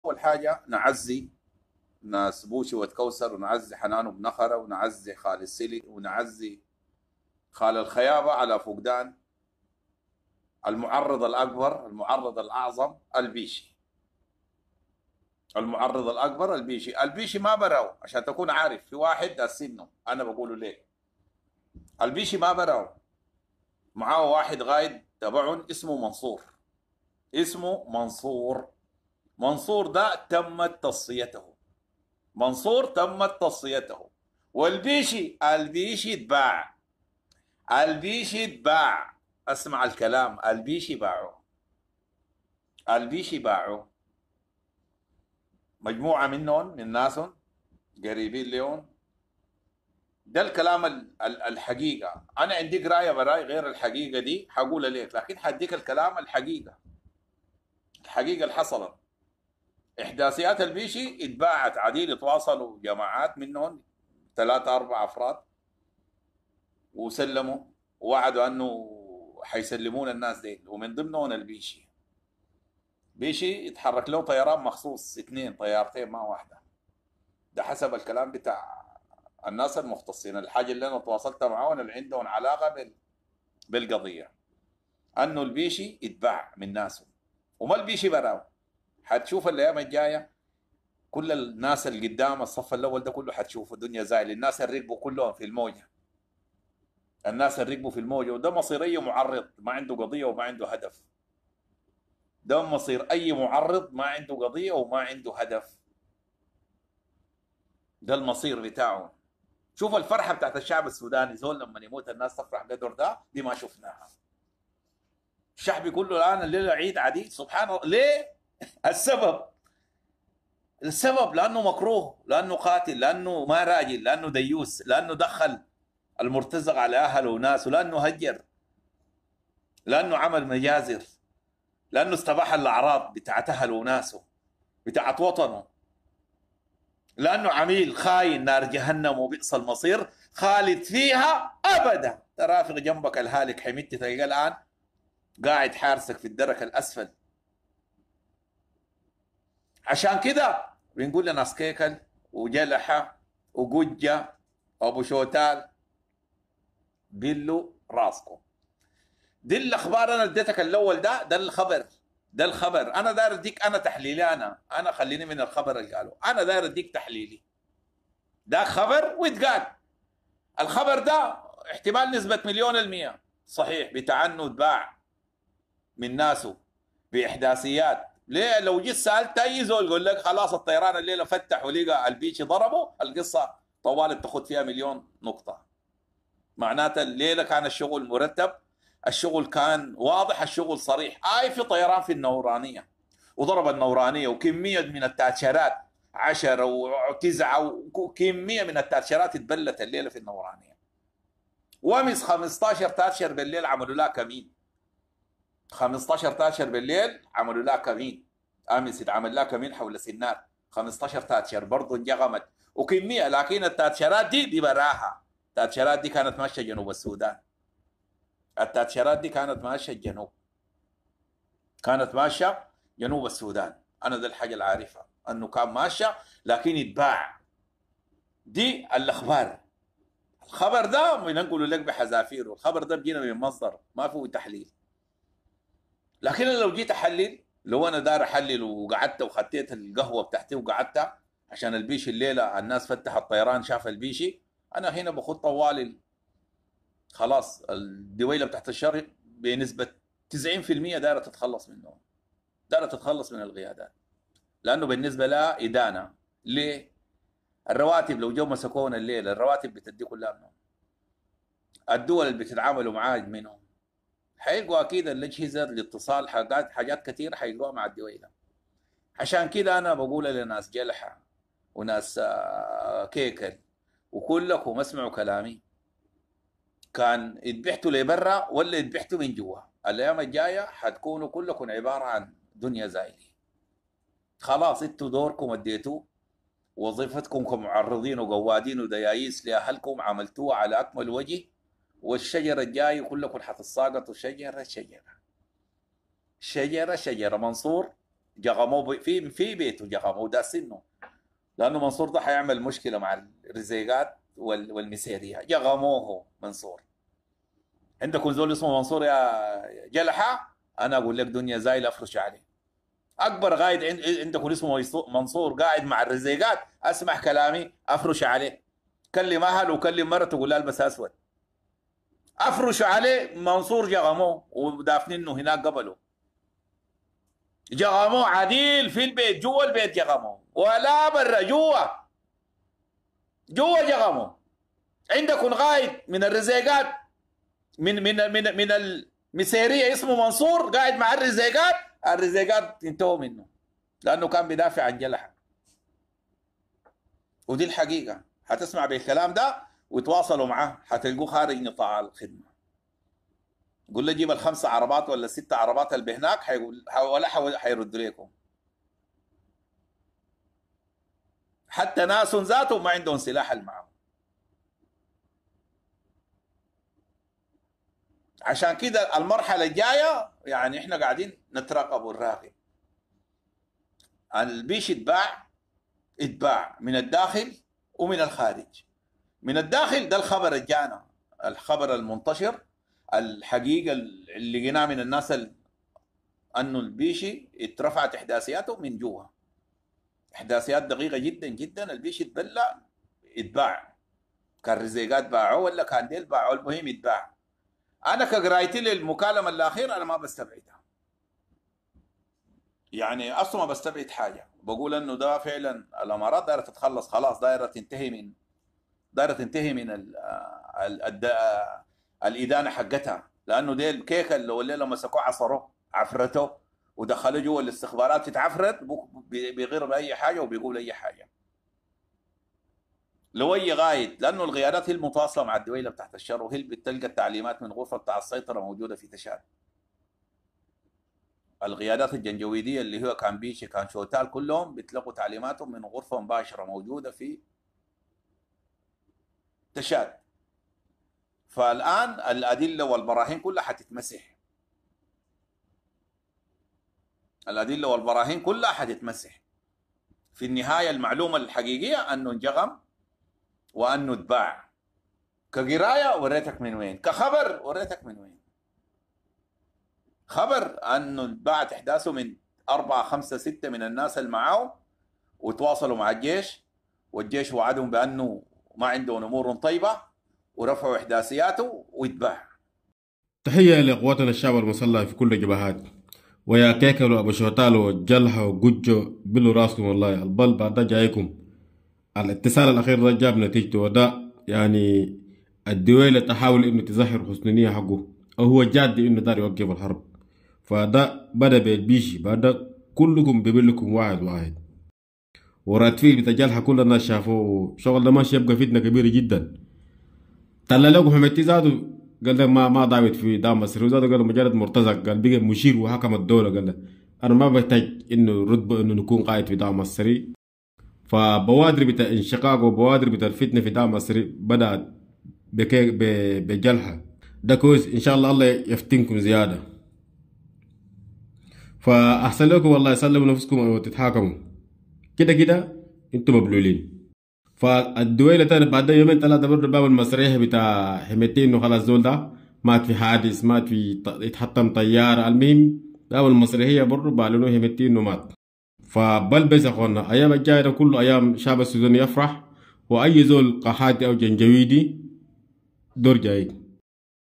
بسيوني/ الزيت من جوه/تفاصيل بيع البيشي و منصور/ دويلة عاوزة تتخلص من الادلة
بسيوني/ الزيت من جوه/تفاصيل بيع البيشي و منصور/ دويلة عاوزة تتخلص من الادلة و البيشي فقط البدايه